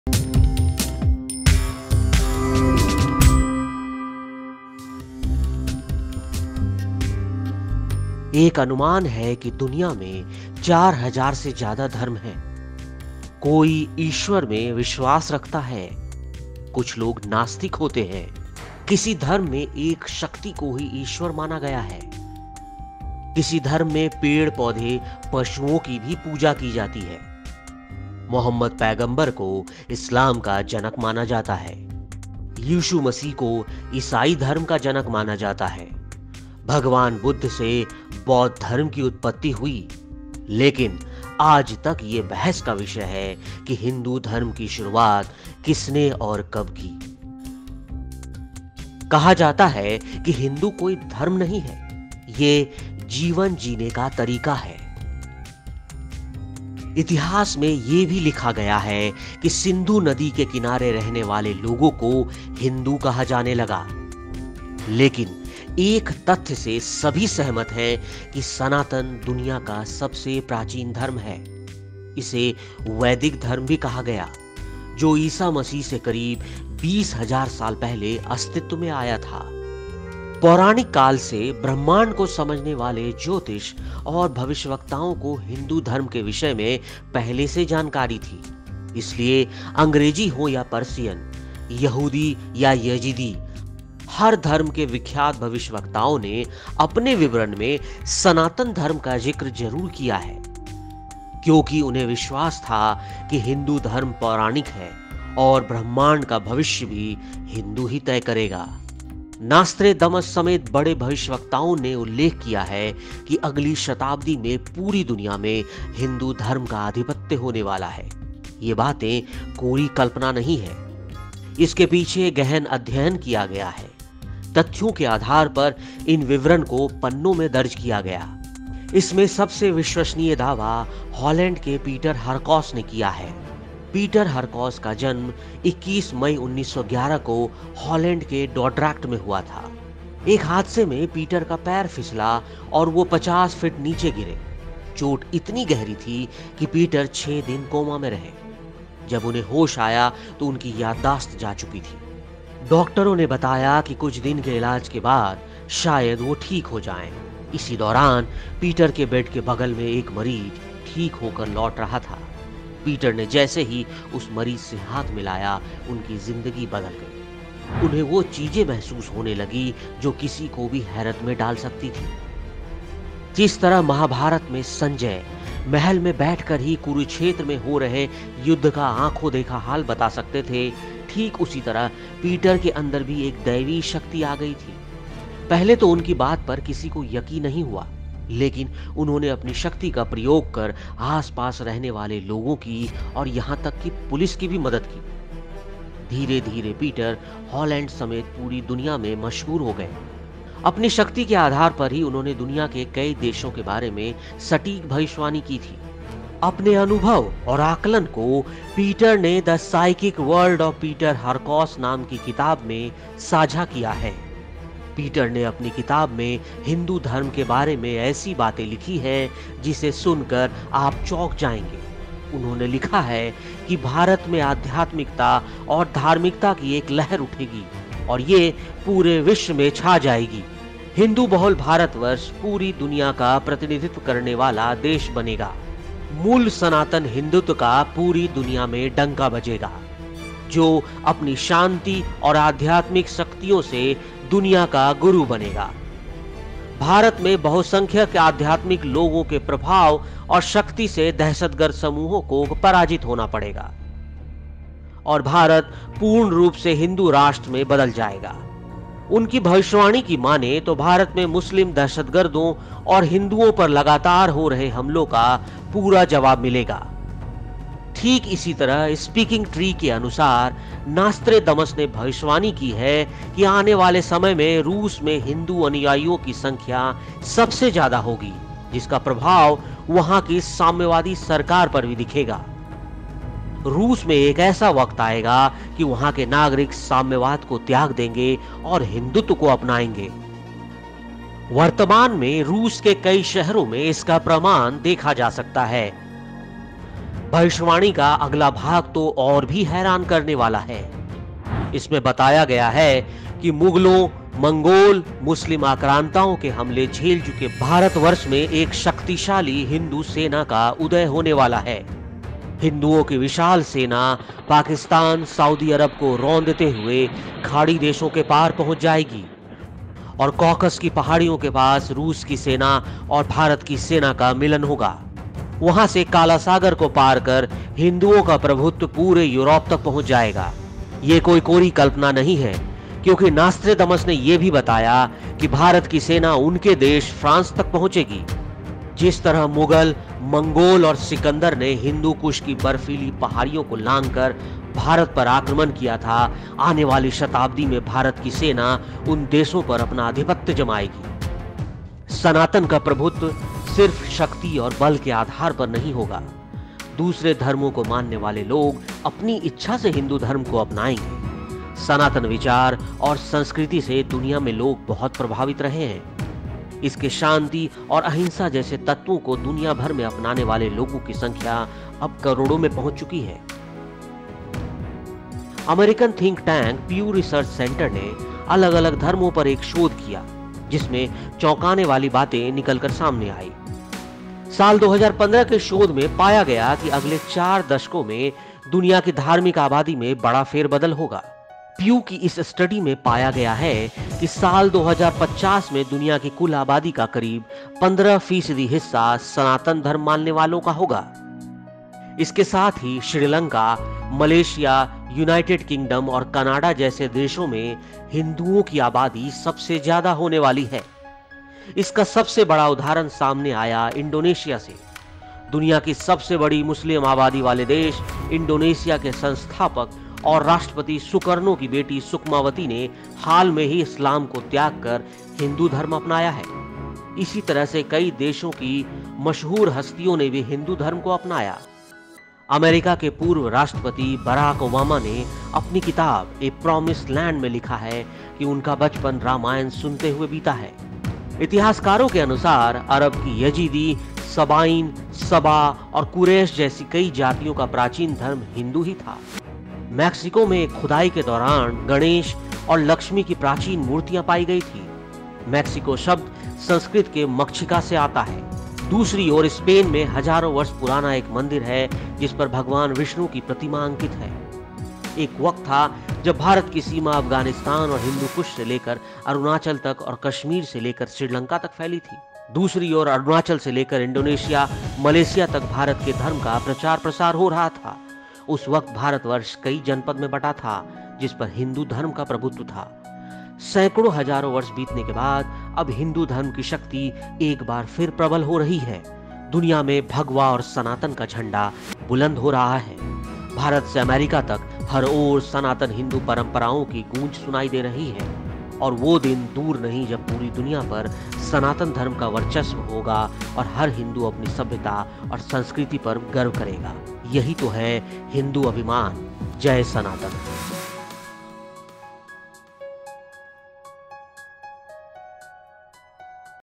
Nostradamus -Peter Hurkos की चौंकाने वाली भविष्यवाणी, Ukraine-Europe का भविष्य बदलेंगे हिंदू ?
The astrologers and prophets who understood the universe from the mythological period already had knowledge about Hinduism ... so be it English or Persian Judaism or Yazidi ... the famous prophets of every religion have definitely mentioned Sanatan Dharma in their description. Because they believed that Hinduism is mythological and that the future of the universe will be decided by the Hindu itself. These things are not pure speculation, a deep study has been done behind it, on the basis of facts, these details have been recorded in the pages.. The most reliable claim in this is made by Peter Harkaus of Europe Holland. Peter Harkaus was born on 21 May 1911 in Drodrecht, Hollande. In an accident, Peter's foot slipped and he fell 50 feet.. The injury was so deep that Peter was in a coma for 6 days. Maybe he'll be fine… Meanwhile, a patient was returning from next to Peter's bed, as Peter shook hands with that patient, his life changed, he began to feel things that could surprise anyone. …just as Sanjaya in Mahabharata… ... but he used his power to help the people living around ... and the police ... gradually Peter became famous all over the world including Holland.. On the basis of his power, he helped many countries of the world. Were predicted about...which have been proving to be true...Peter has shared his experience and assessment in a book called The Psychic World of Peter Hurkos….
Peter has written such things about Hinduism in his book that you will be shocked to hear… he has written that
'A wave of spirituality and religiosity will rise in India and will cover the whole world.' The Hindu majority India will become a country representing the whole world. The original Sanatan Hindutva will ring in the whole world, which will become the master of the world with its peace and spiritual powers…. groups will have to be defeated by the influence and power of majority of spiritual people in India..

According to Speaking Tree, Nostradamus has predicted that in the coming times, Russia will have the largest number of Hindu followers, whose effect will also be seen on the communist government there ... a time will come in Russia that the citizens there will give up communism and We will adopt Hindutva... At present, evidence of this can be seen in many cities of Russia.
The next part of the prophecy is even more surprising ... it has been told that a powerful Hindu army is going to rise in India, which has faced the attacks of Mughals, Mongol, Muslim invaders Huge army of Hindus Pakistan, Saudi Trapping Arabia will reach across the Gulf countries... Near the Caucus hills the Russian army and the Indian army will meet, and from there the Hindu domination will reach Europe by crossing the Black Sea. This is not a figment of imagination because Nostradamus also told that Aryavarta's army would reach his country France and europe. Just as the Mughals... Mongols.. Alexander invaded India by crossing the icy mountains of Hindukush. The supremacy of Sanatani will not be on the basis of strength and force alone.
 People following other religions...will adopt Hinduism at will.
People in the world have been greatly influenced by Sanatan thoughts and culture. The number of people who have adopted its elements like peace and non-violence around the world has now reached into crores. American think tank Pew Research Center did a research on different religions, in which many surprising things came out. Research from the year 2015 found that the world's religious population will undergo major changes in the next four decades. In this study of Pew, it was found that in the year 2050, about 15 percent of the total population of the world will be the followers of Sanatan Dharma. At that time there would be maximum number of Hindus in India. Along with this, the population of Hindus is going to be highest in countries like Sri Lanka, Malaysia, United Kingdom and Canada. The biggest example of this came
Sukmavati, the daughter of Sukarno, the founder and first president of Indonesia, the country with the world's largest Muslim population, has renounced Islam and converted to Hinduism. Similarly, celebrities of many countries also adopted Hinduism .. Former US President Barack Obama has written in his book 'A Promised Land' that his childhood was spent listening to Ramayana. Many American actors like Julia Roberts have also adopted Hinduism. According to historians, the ancient religion of many castes like Yazidi, Sabine, Sabah, Quraysh etc. of Arabia was Hindu.